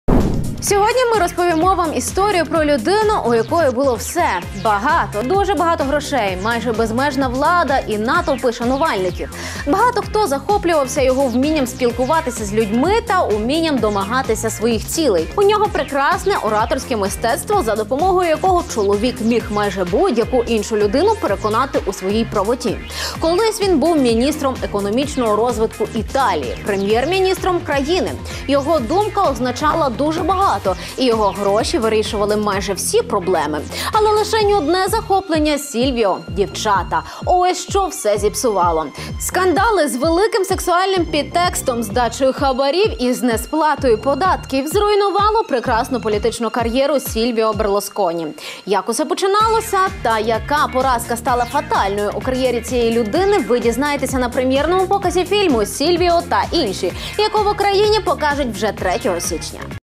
Скоро! Прем’єра фільму «Сільвіо та інші». ПравдаТУТ Львів
Сьогодні ми розповімо Вам історію про людину, у якої було все – багато, дуже багато грошей, майже безмежна влада і натовпи шанувальників. Багато хто захоплювався його вмінням спілкуватися з людьми та умінням домагатися своїх цілей. У нього прекрасне ораторське мистецтво, за допомогою якого чоловік міг майже будь-яку іншу людину переконати у своїй правоті. Колись він був міністром економічного розвитку Італії, Прем'єр-Міністром країни. Його думка означала дуже багато. І його гроші вирішували майже всі проблеми. Але лишень одне захоплення Сільвіо – дівчата - ось що все зіпсувало. Скандали з великим сексуальним підтекстом, з дачею хабарів і з несплатою податків – і зруйнувало прекрасну політичну кар’єру Сільвіо Берлусконі. Як усе починалося та яка поразка стала фатальною у кар’єрі цієї людини Ви дізнаєтеся на прем’єрному показі фільму «Сільвіо та інші», яку в Україні покажуть вже 3-го січня!